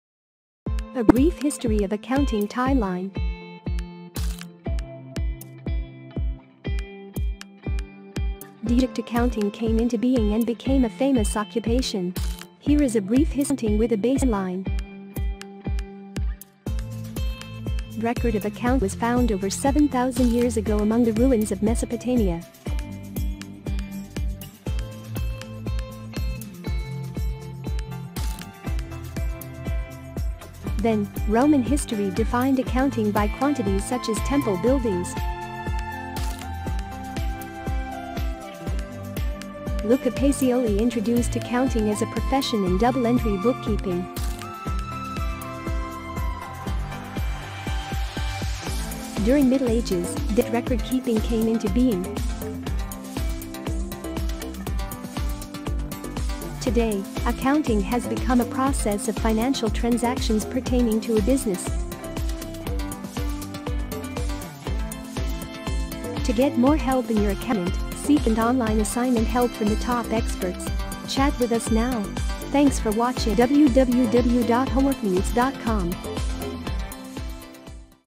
A Brief History of Accounting Timeline.
Do you know how subject accounting came into being & became a famous occupation.
Here is a brief history of accounting with a basic timeline.

The earliest record of Accounting was found over 7,000 years ago among the ruins of Mesopotamia.
Then, Roman history defined accounting by quantities such as temple buildings.
Luca Pacioli introduced accounting as a profession in double-entry bookkeeping.
During middle ages, debit & credit record keeping came into being.
Today, accounting has become a process of financial transactions pertaining to a business.

To get more help in your accounting assignment, seek instant online assignment help from the top-experts. Chat with us now.